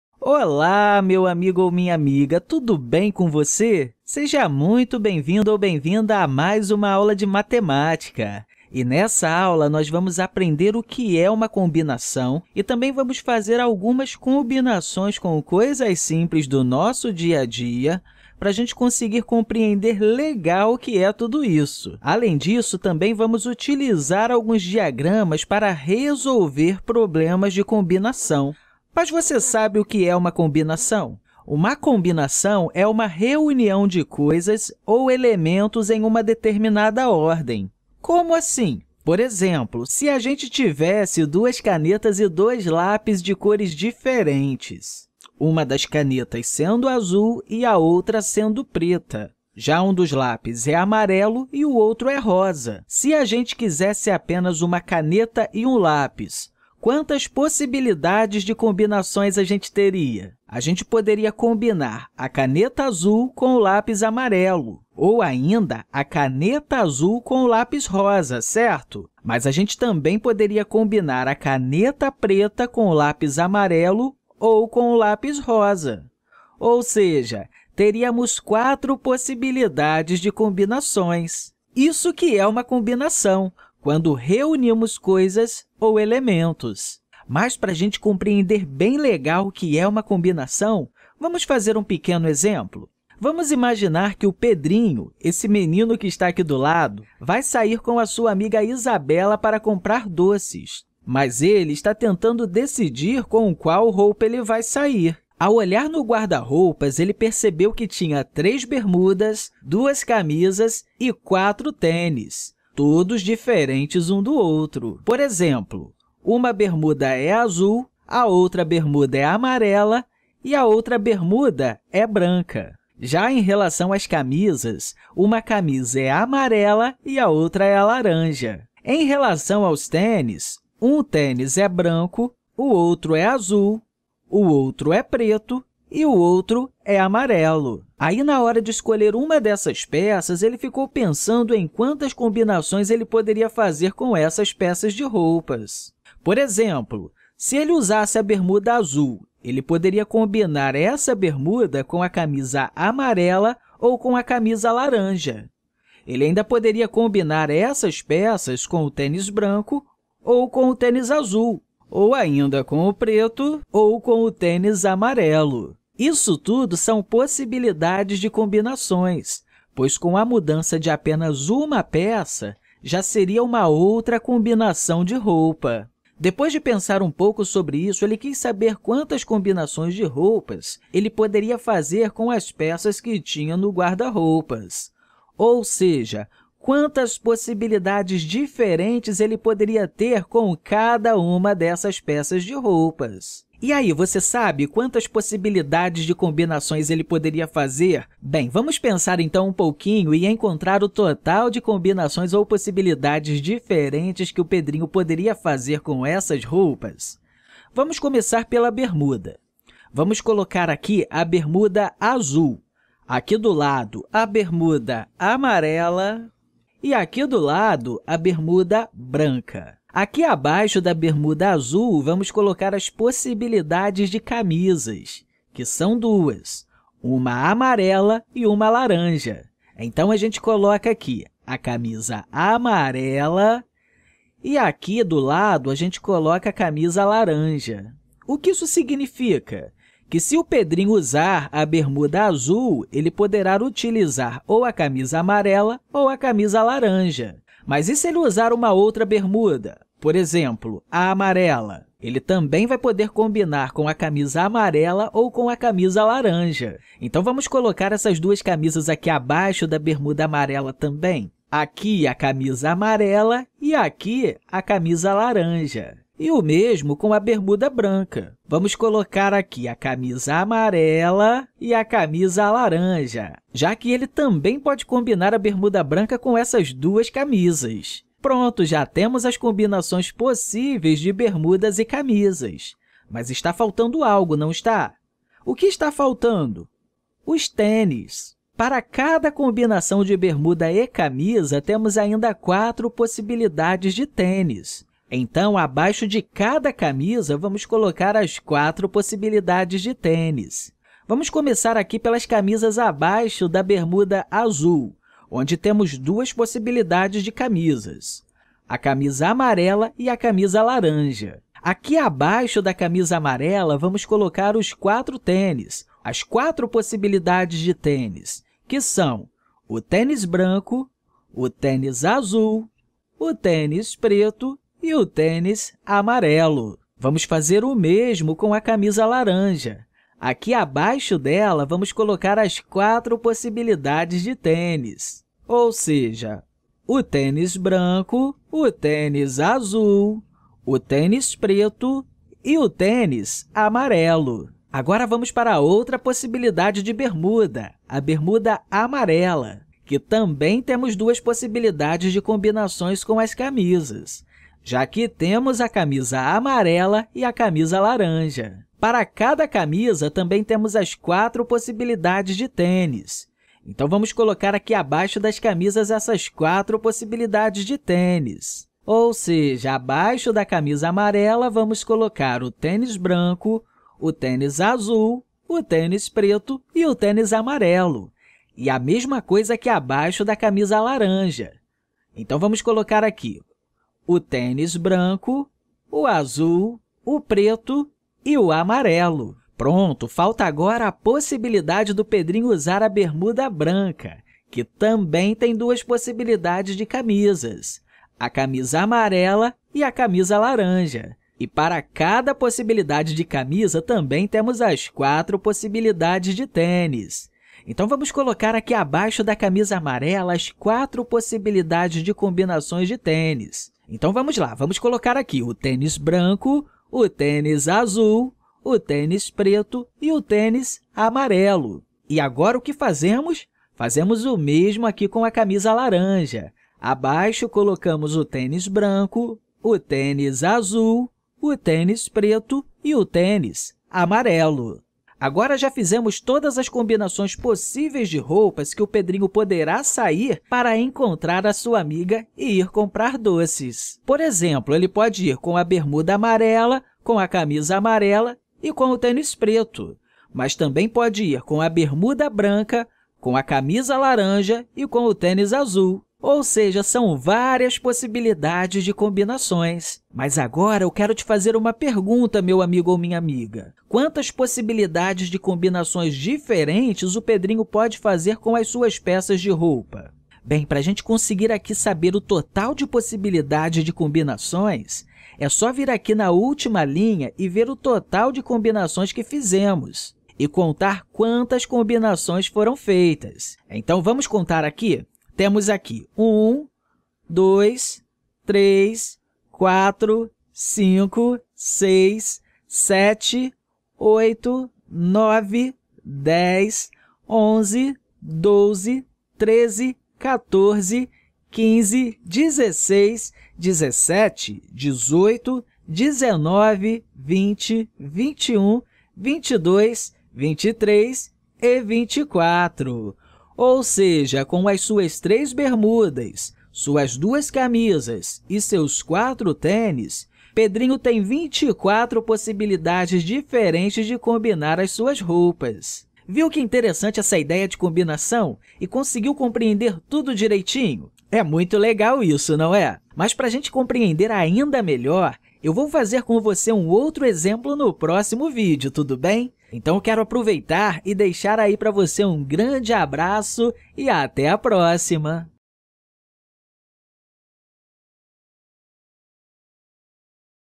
Uso de diagramas para resolução de problemas de combinação | Parte I
Nessa videoaula você irá aprender a fazer combinações de problemas simples utilizando diagramas como forma de estratégia